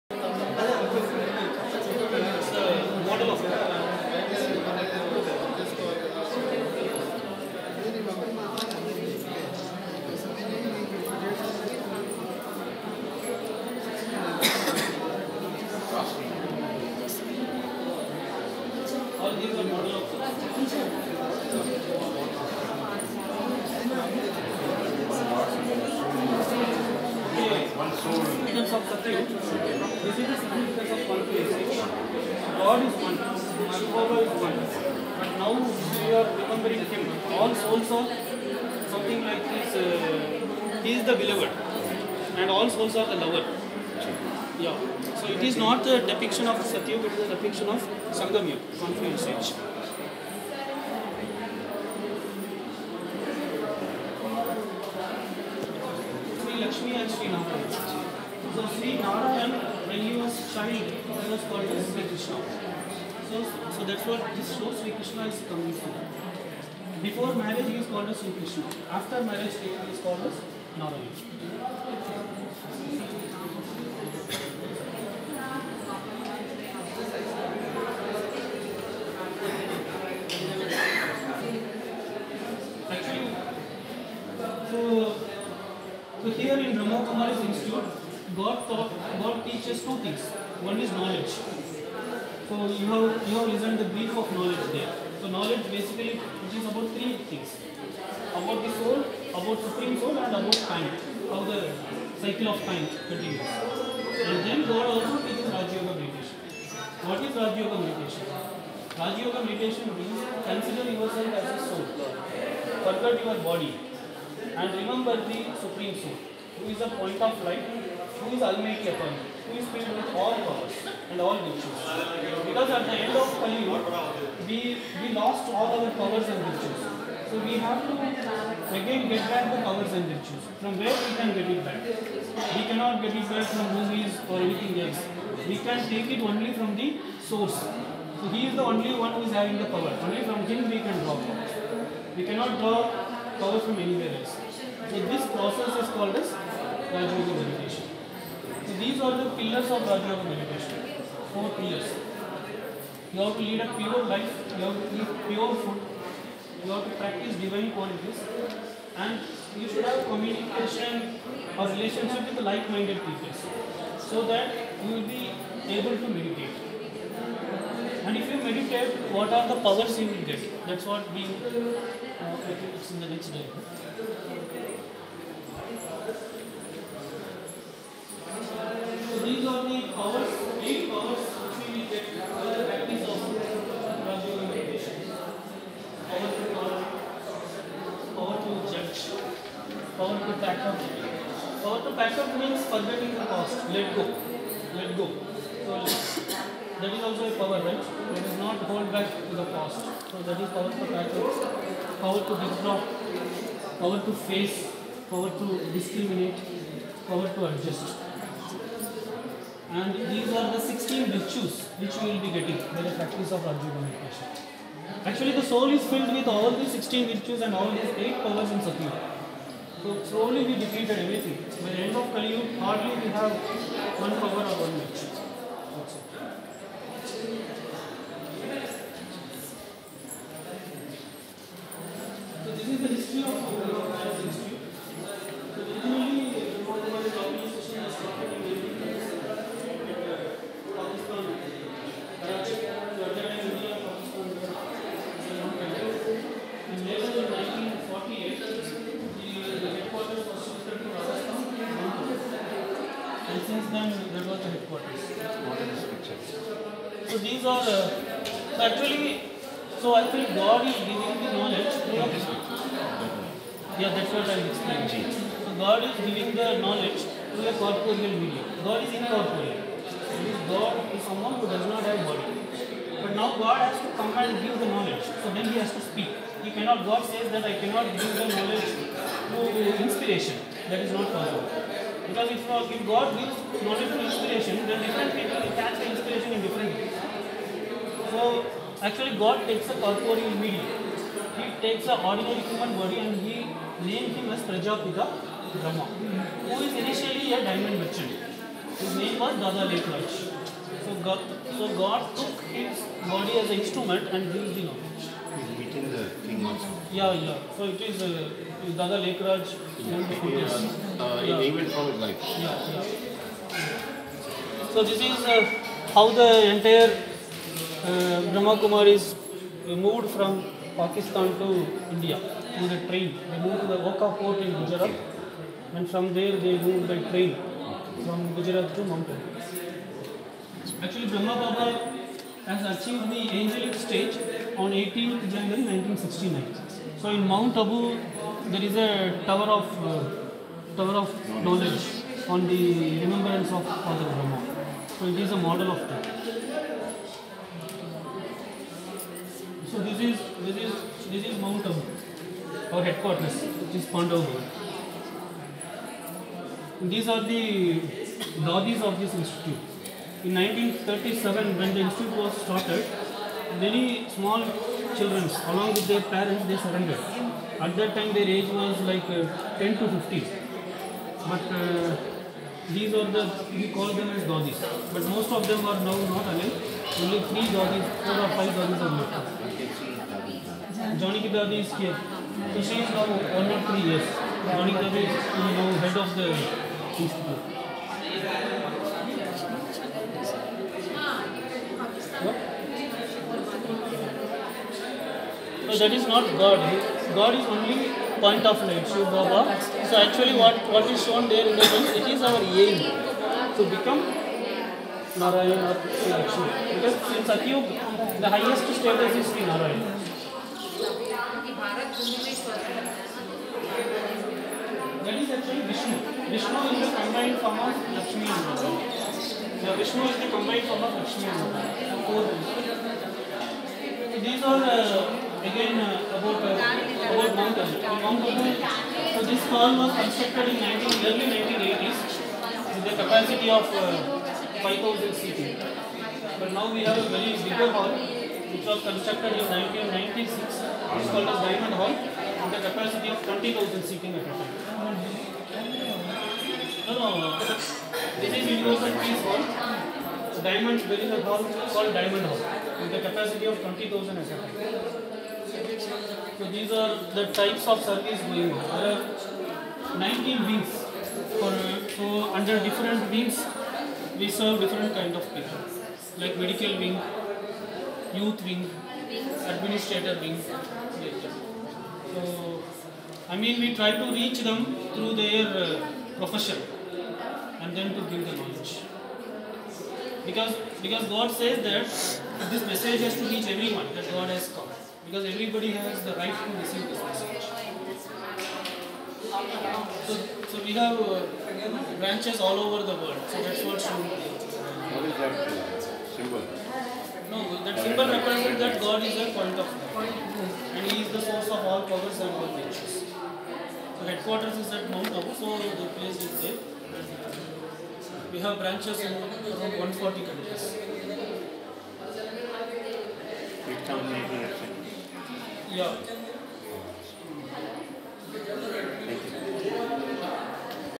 Brahma Kumaris Hyderabad,Spiritual Art Gallery and Museum at Brahma kumaris Shanti sarovar Hyderabad
Shanti Sarovar, Brahma Kumaris Hyderabad ,Spiritual Art Gallery and Museum at Hyderabad Brahma kumaris Shanti sarovar.

A very beautiful and Enlightening Spiritual Art Gallery and Museum at Hyderabad Brahma kumaris Shanti sarovar.